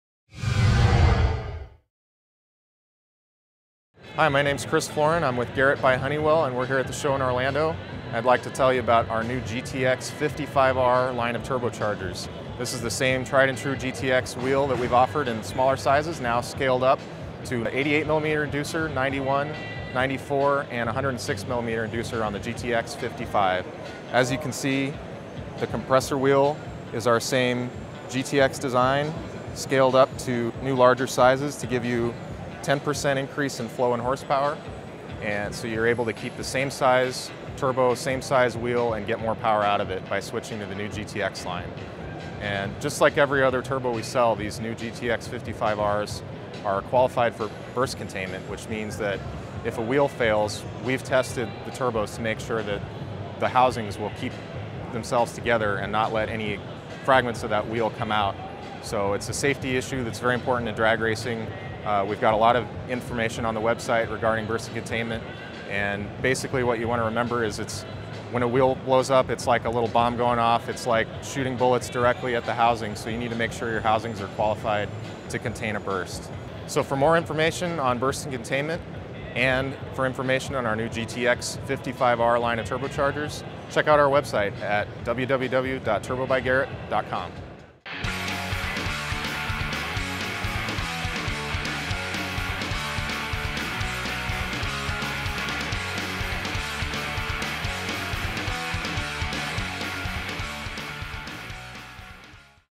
Garrett by Honeywell
Chris Floren from Garrett by Honeywell introduces the company's new GTX 55R line of turbochargers, scaled up to larger sizes.